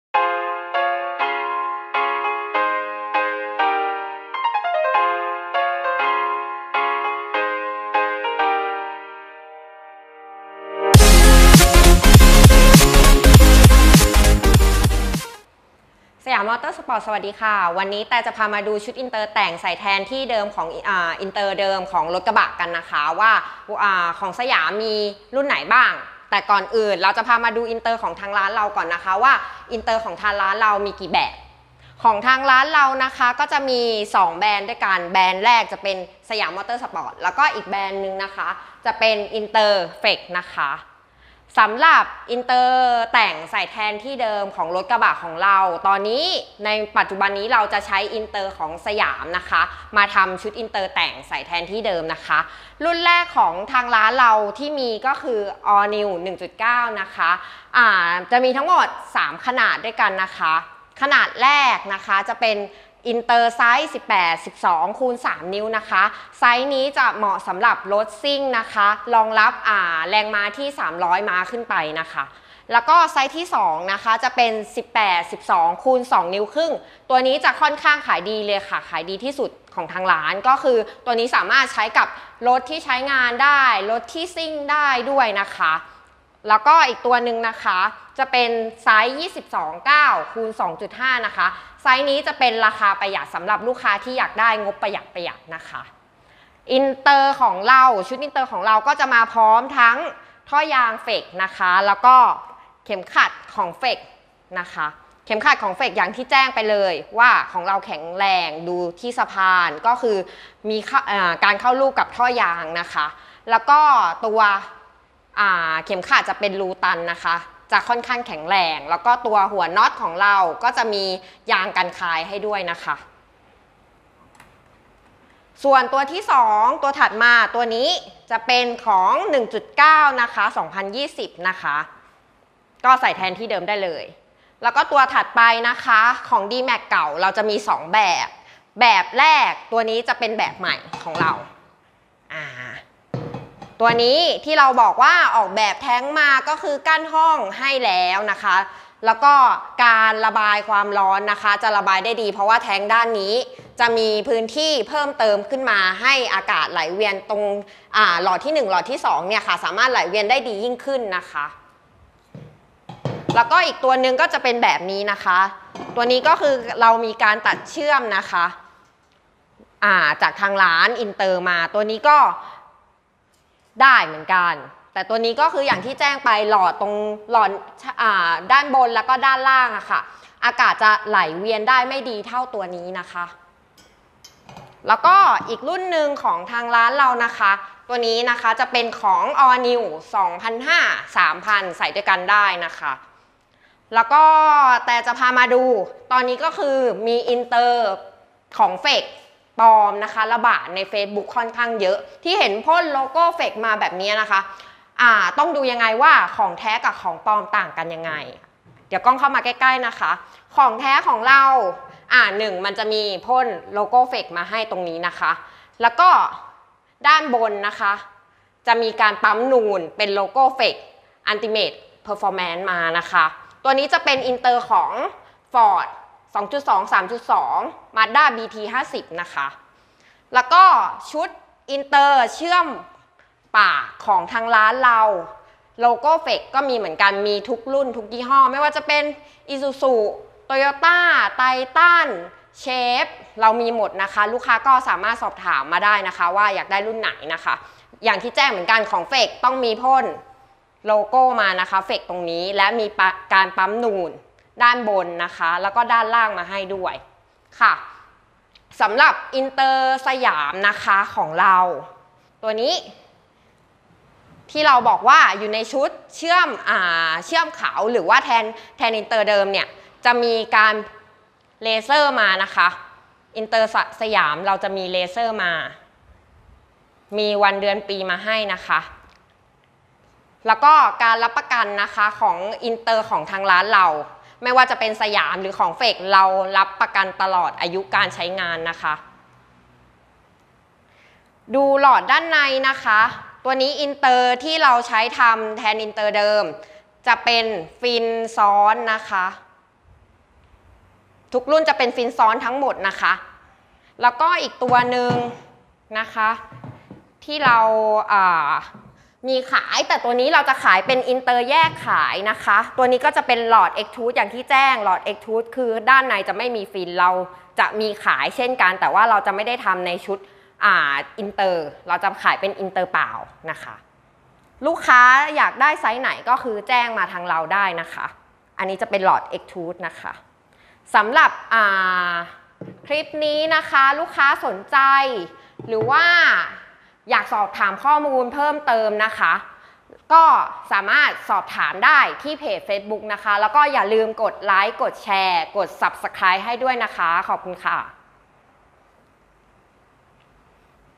อินเตอร์แต่งใส่แทนของเดิมได้เลย สำหรับกระบะทุกรุ่น
เริ่มต้น เพียง 2,800 บาท เท่านั้น
ISUZU DMAX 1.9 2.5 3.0
FORD RANGER / BT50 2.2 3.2
สินค้าคุณภาพรับประกัน อินเตอร์แตก บวม รั่ว ตลอดอายุการใช้งาน

📣 ร้านเปิดบริการ จ-ส 📷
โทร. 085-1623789
📌ร้านตั้งอยู่ที่ อ่อนนุช ถ.สุขาภิบาล2